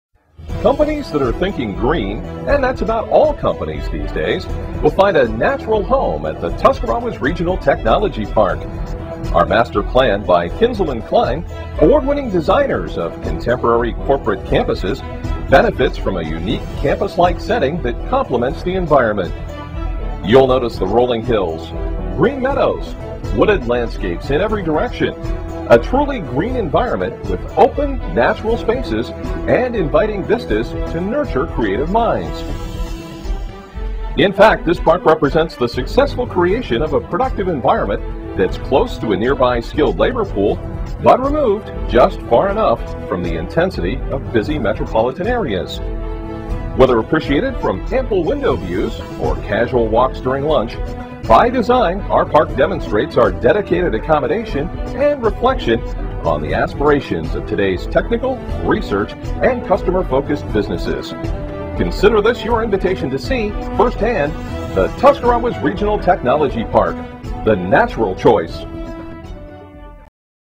Technology Park Overview
Watch this video to learn why you should locate your business in the Tuscarawas County Ohio Technology Park.